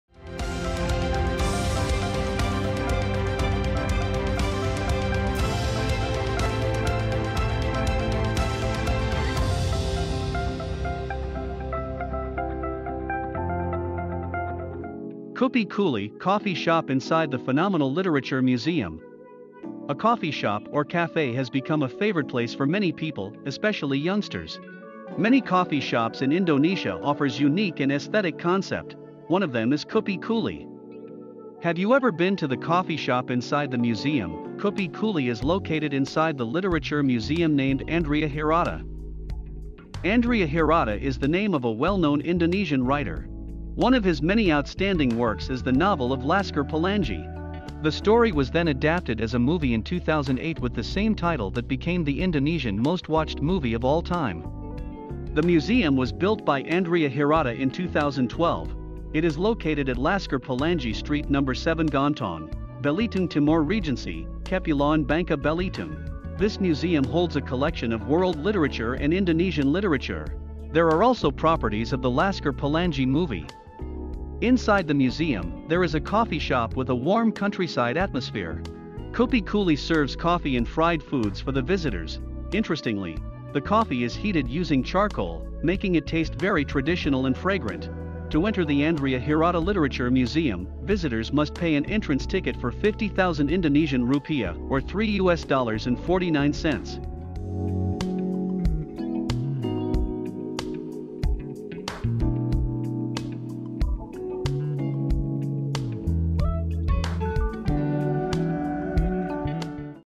Kupi Kuli, Coffee Shop inside The Phenomenal Literature Museum
A coffee shop or cafe has become a favorite place for many people, especially youngsters.  Many coffee shops in Indonesia offers unique and aesthetic concept.

One of them is Kupi Kuli.

Have you ever been to the coffee shop inside the museum? Kupi Kuli is located inside the Literary Museum named Andrea Hirata.

Voice Over / Video Editor: Aulia Hafisa / Praba Mustika